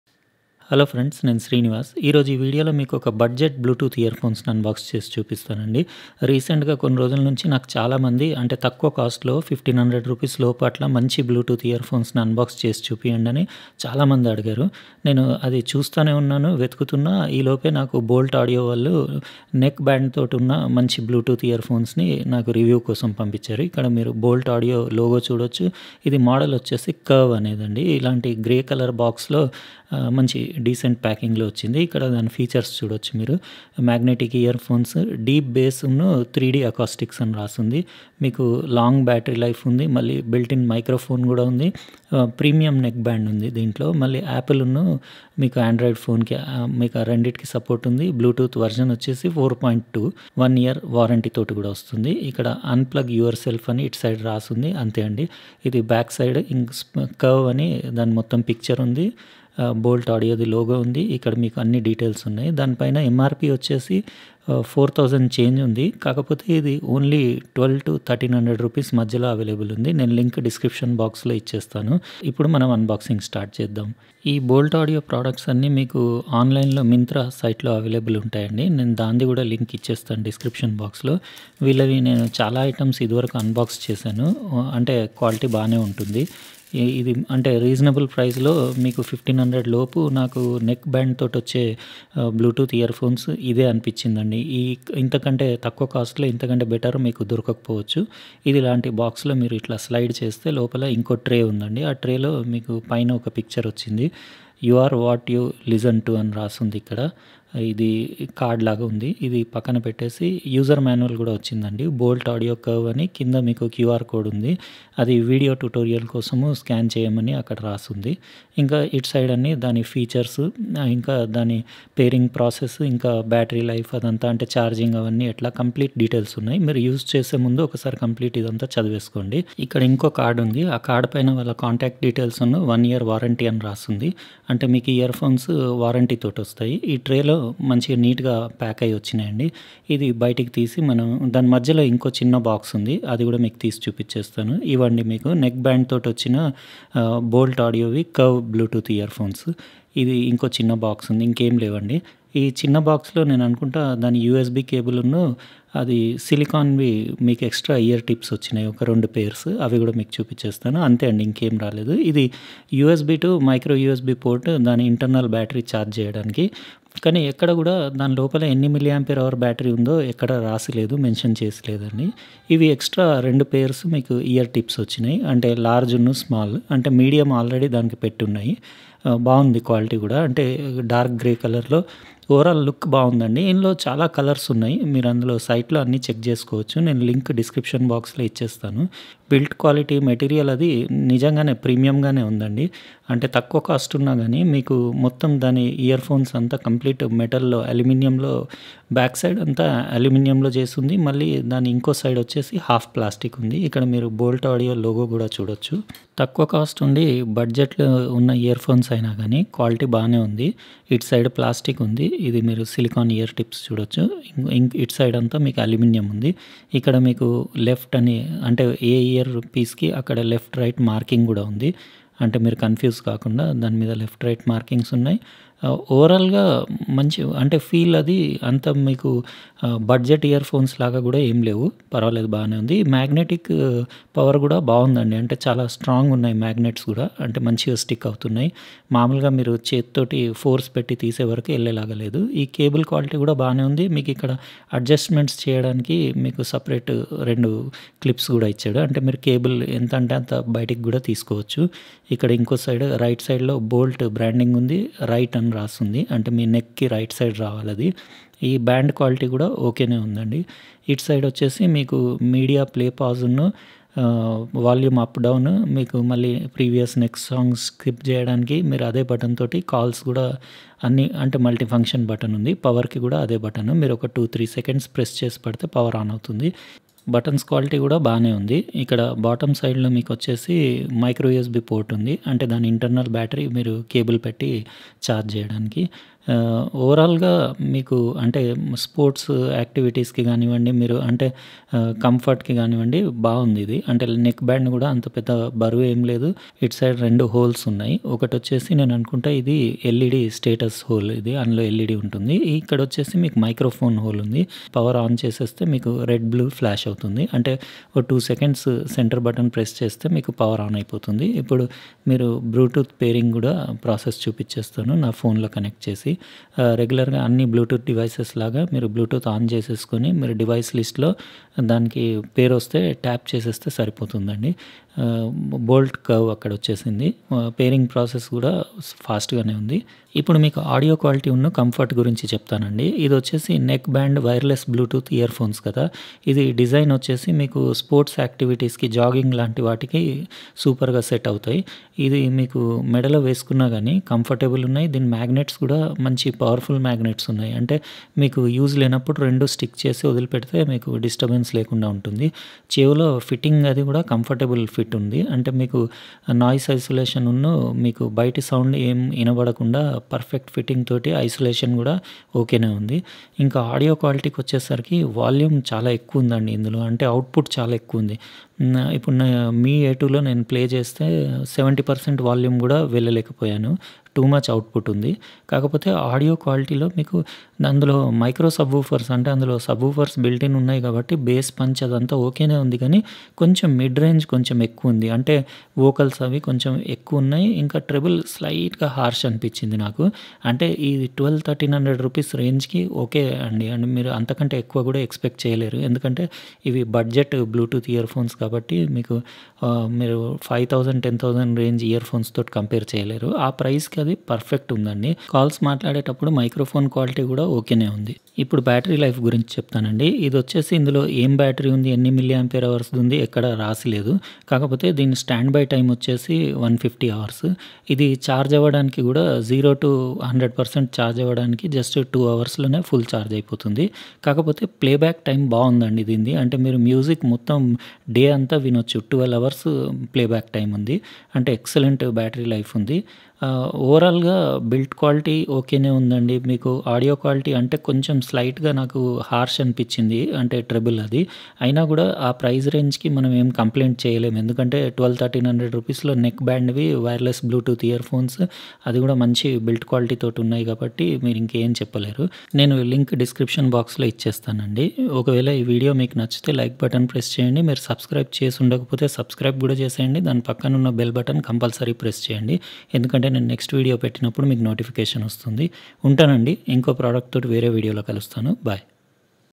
Boult Audio Curve Neckband Earphones Unboxing & Review in Telugu
Boult Audio Curve Neckband Budget Magnetic Bluetooth Earphones Unboxing & Review in Telugu | Boult Audio Curve Wireless Earphones Unboxing & Review in Telugu.

Srinivas Chintapatla